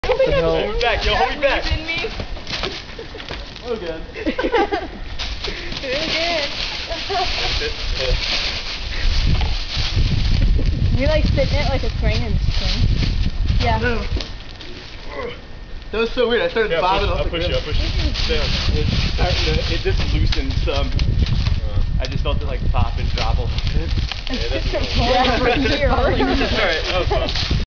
Swinging on a vine.
Having fun with a vine (until it comes loose)